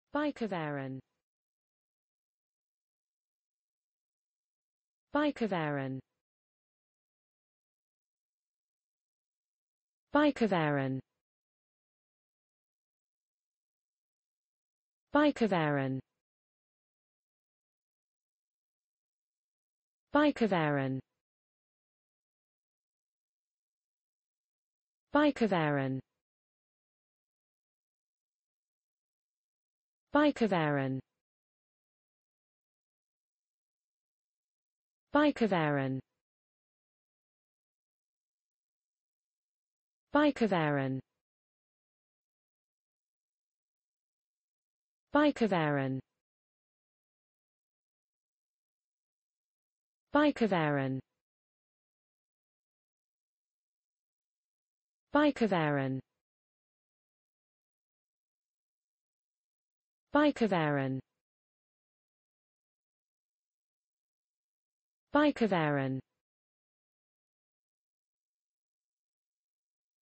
bikaverin: How to pronounce bikaverin in english (correct!).Start with B. Learn from me.
Follow me!Learn from me how to pronounce the word bikaverin in english.This video shows you how to pronounce the word bikaverin in english.

bikaverin

n. 比卡菌素